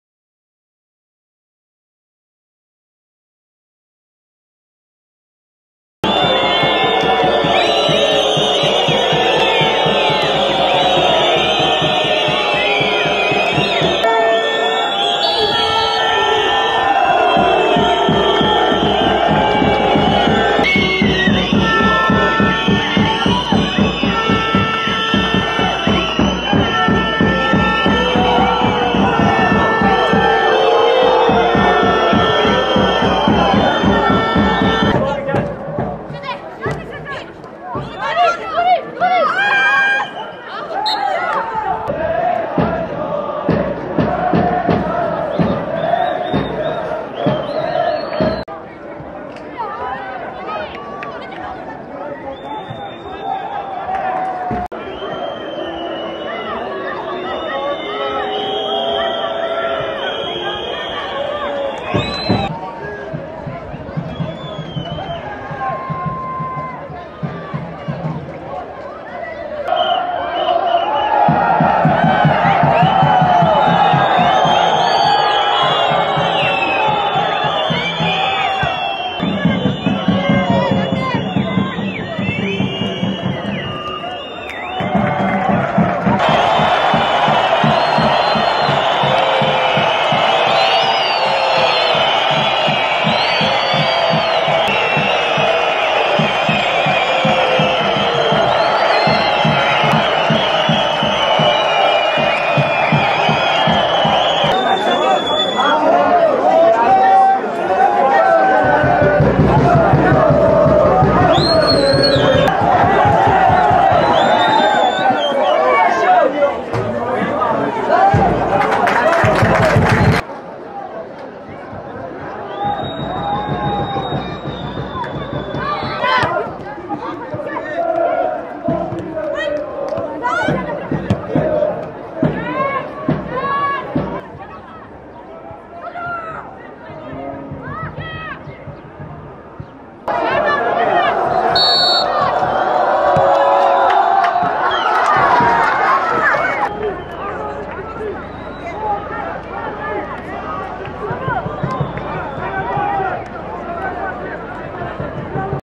Hakkarigücü - Fenerbahçe : 1-1​​​​​​ | Futbol Turkcell Kadın Süper Ligi play-off
GÖRÜNTÜ DÖKÜMÜ :
- Takımların sahaya çıkması
- Maç içi mücadele
- Futbolcuların gol sevinci
- Taraftarların takımlarını desteklemesi
- Bazı vatandaşların müsabakayı yüksek noktalardan takip etmesi
- Futbolcuların birbirini tebrik etmesi Futbol: Turkcell Kadın Süper Ligi play-off
- Hakkarigücü: 1 - Fenerbahçe: 1
İSTANBUL (AA) - Turkcell Kadın Futbol Süper Ligi play-off birinci tur ilk maçında Hakkarigücü ile Fenerbahçe 1-1 berabere kaldı.
Ligde A Grubu'nu zirvede tamamlayan Fenerbahçe ile B Grubu'nu 3. sırada bitiren Hakkarigücü karşı karşıya geldi.
Merzan Şehir Stadı'nda oynanan karşılaşmada Hakkargücü 34. dakikada Süheyla Çalçınar'ın golüyle 1-0 öne geçerken, ilk yarı da bu skorla tamamlandı. Fenerbahçe maçın 71. dakikasında Kennya Cordner'ın golüyle 1-1 beraberliği sağladı ve karşılaşma 1-1 beraberlikle tamamlandı.

İki takım arasındaki rövanş karşılaşması ise 21 Mayıs Cumartesi günü saat 16.00'da İstanbul'da oynanacak.